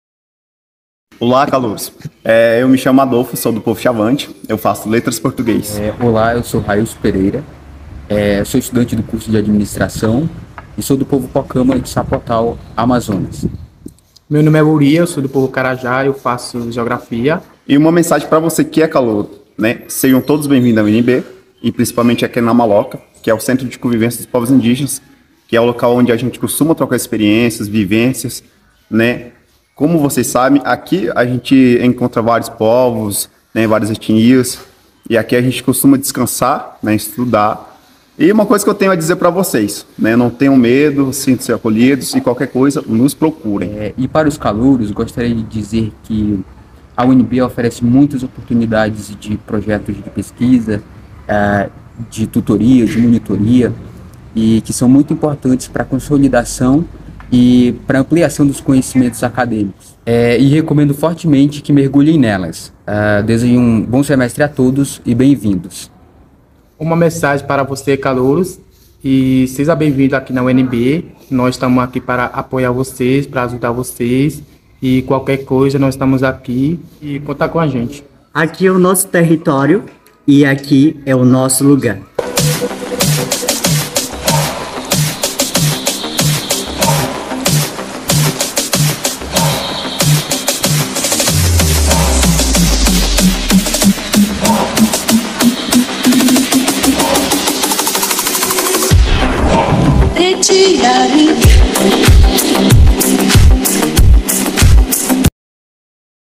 Uma mensagem para os calouros com os Estudantes Indígenas da Universidade de Brasília
Participação dos Acadêmicos Indígenas

Adolfo - Estudante do curso de letras e Português
Railso Pereira - Estudante do curso de administração
Wouria - Estudante do curso de Geografia.